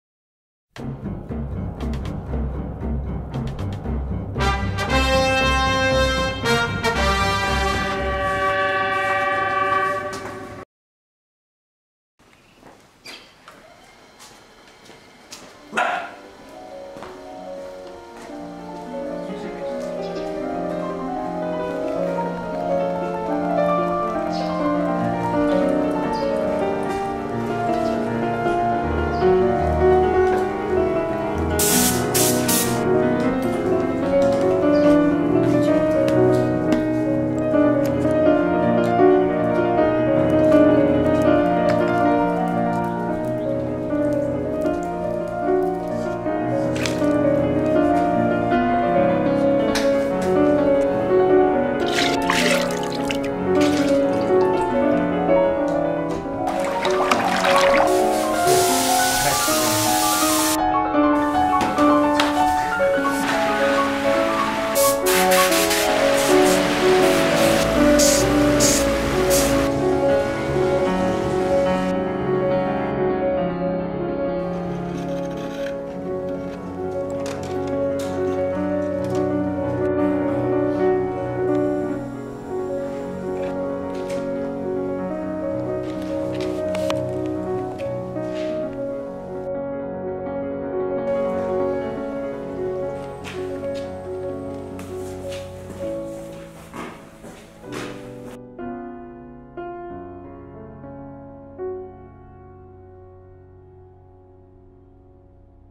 Persimmon Tea/Candies/Coffee Beans Jar Handmade Procedure
Ceramics and porcelains products in Glorious Collection are mostly handmade products which carefully made under the following 14 procedures: materials selecting, kneading mud, shaping, drying in shade, triming, biscuiting,paint under the glaze, glazing, load in kiln, firing, taking out from the kiln, decoration firing, finished work.